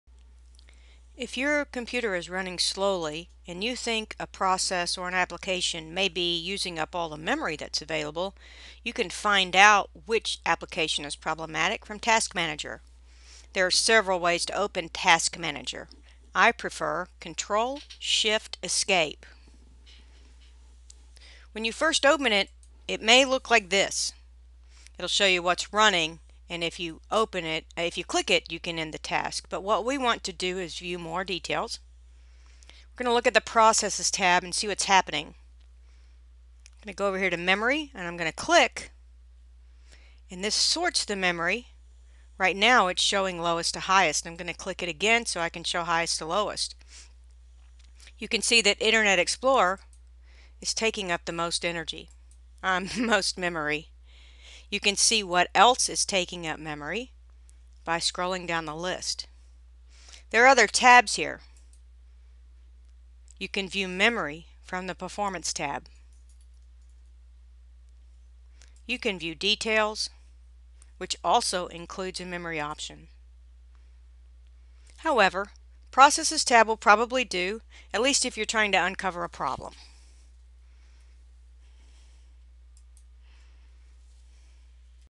Windows 8.1: UseTask Manager to See What Applications are using the Most Memory
Memory, or RAM, is extremely important to computer performance. If a single application is using a lot of memory, you'll notice. You can use Task Manager to list running applications by how much memory they use to uncover problems.